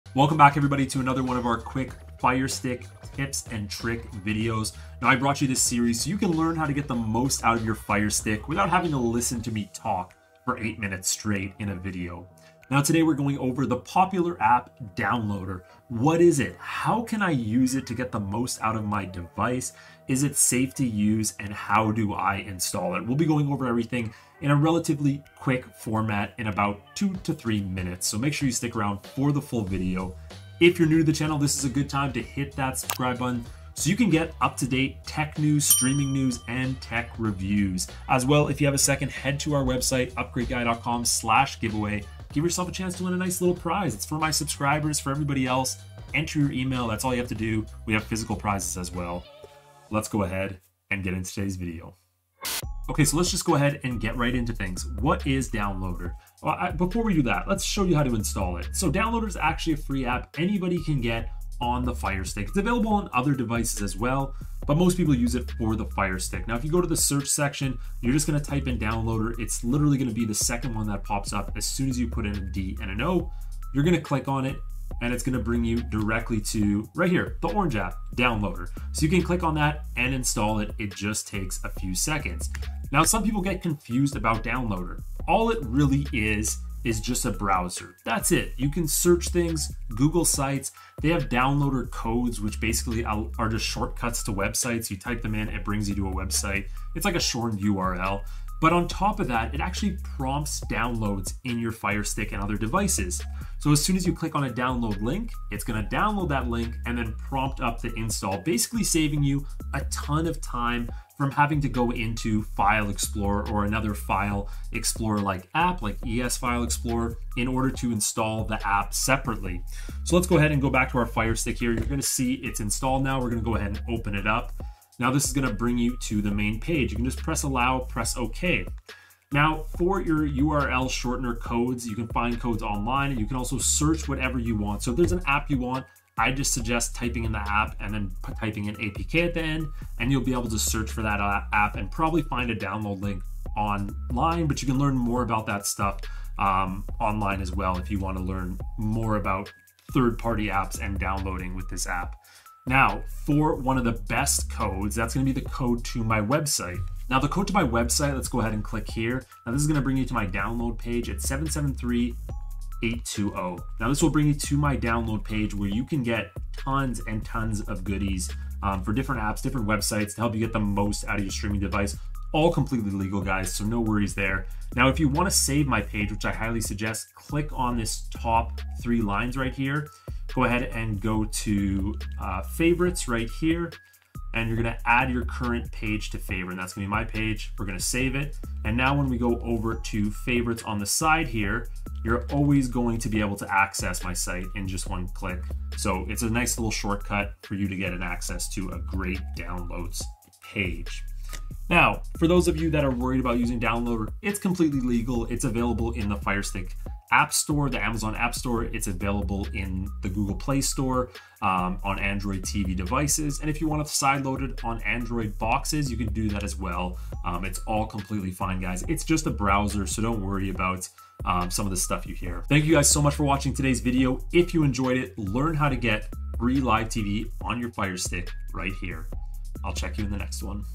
Downloader On Firestick - What is it and how to use it
Downloader is a popular app that allows users to quickly install application with the click of a button. It essentially takes away the need for file explorer apps. Instead this easy to use browser pops up downloads as soon as they complete in the browser. Users may not fully understand how it works so in today's video I will show you!

📺 Additional videos to help you with breaking news on streaming devices in 2022!